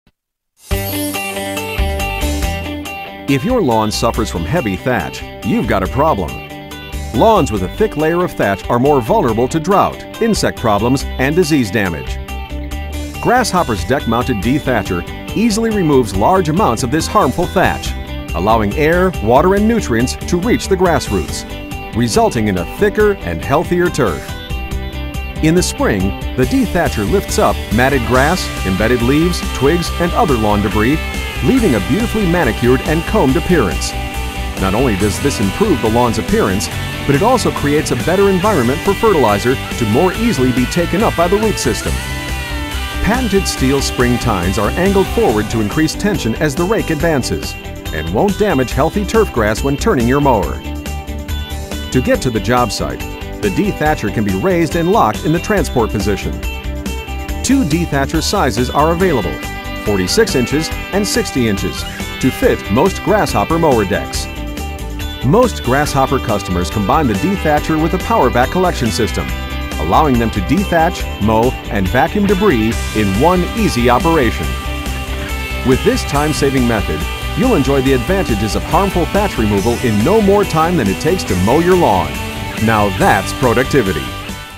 Dethatching & Collecting | Grasshopper Mowers
Unified raking motion removes large amounts of thatch and surface aerates the soil for better root development. Tine-Rake Dethatchers feature spring steel tines that are angled forward to increase tension as the rake advances. Units connect directly to the deck and can be lifted from the operator's seat. Combine with a PowerVac Collection System for efficient, one-step cleanup.